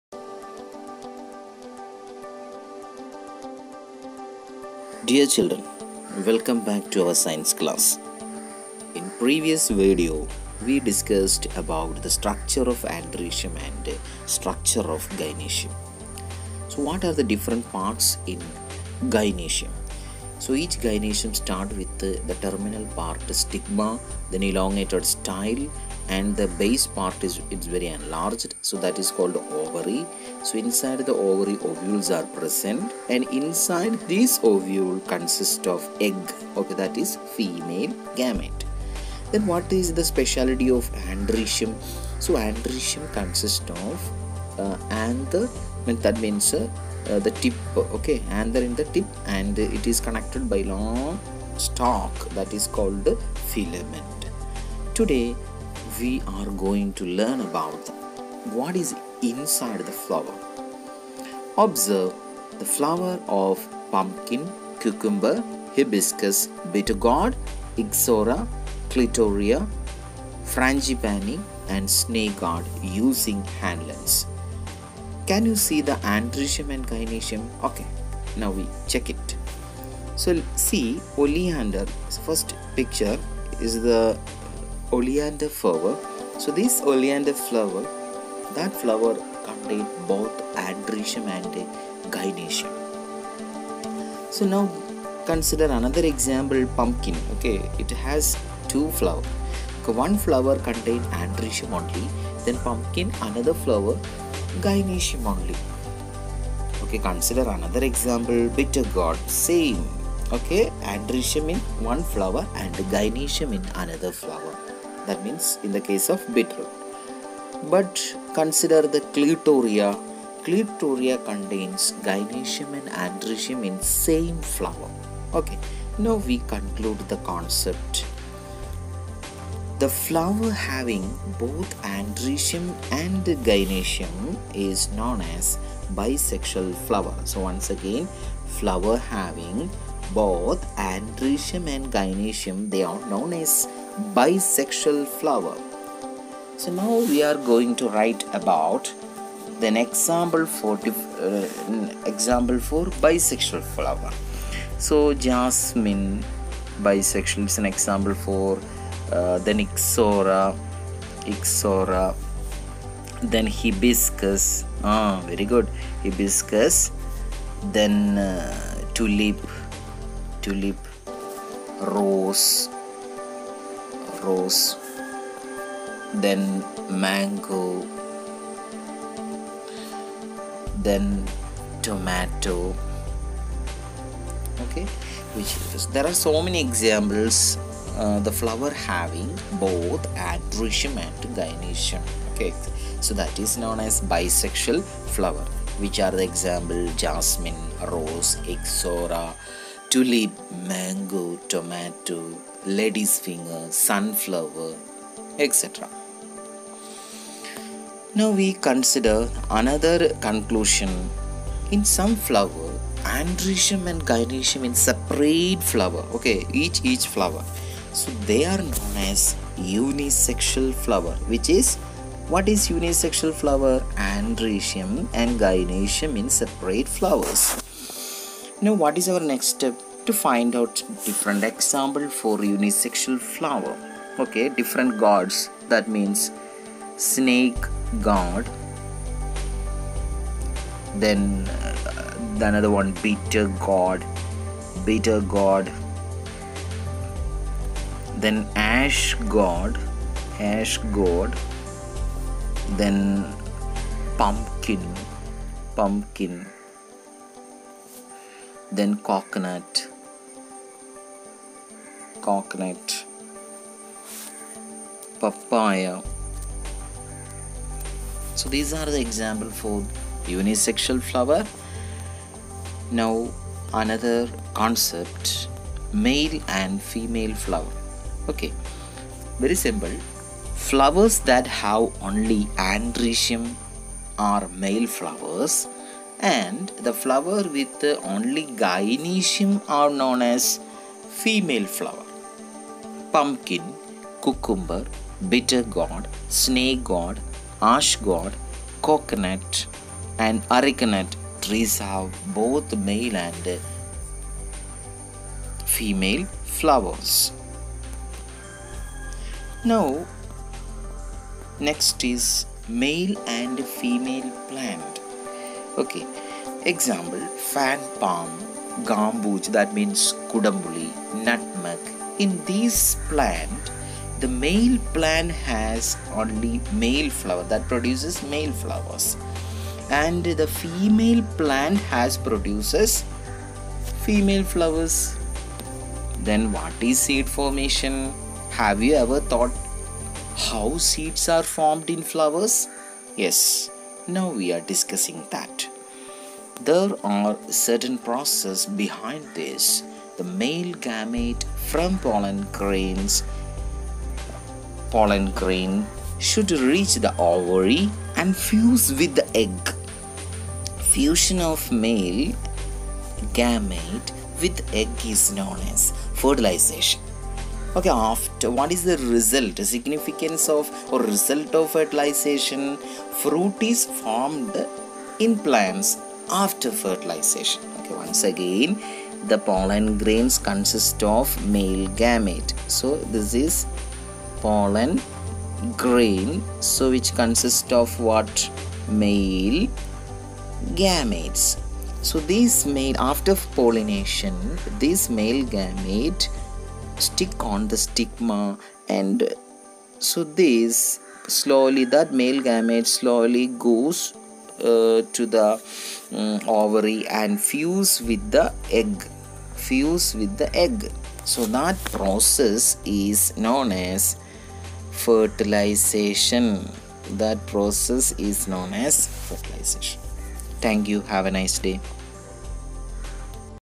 6std BASIC SCIENCE || CHAPTER 3|| Flower to flower,🌺
part-5 ( male Flower,female flower,make plant ,female plant and fertilization)